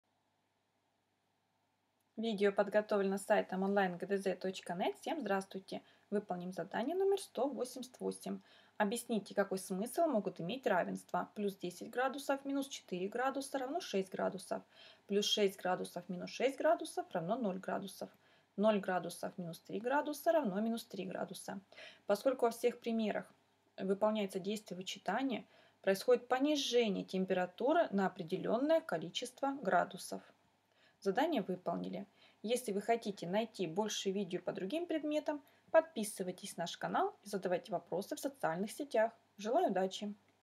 Задание № 188 - Математика 6 класс (Зубарева, Мордкович)
Чтобы сделать задание № 188, в котором надо описать, какой смысл имеют выражения, нужно знать изменение температуры на термометре. Чтобы сделать это задание без ошибок, воспользуйтесь нашим видео с решением.